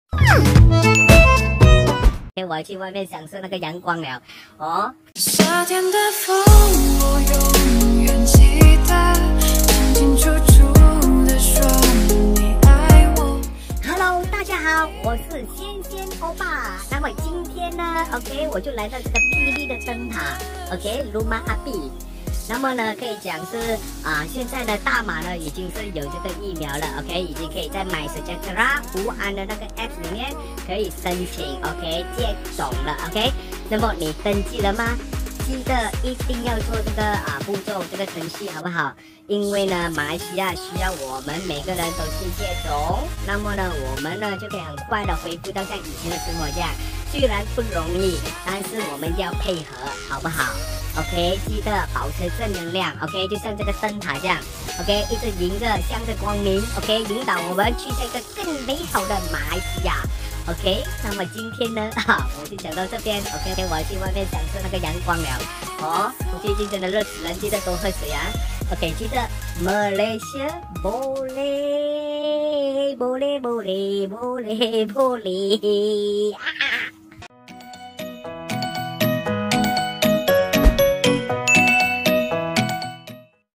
灯塔请指引鲜鲜oppa方向
山穷水尽疑无路，柳暗花明又一“灯”！

大马开始接种疫苗，鲜鲜OPPA走上山，看着灯塔，鼓励大马人勇敢接种！

让我们一起踊跃登记，共创未来！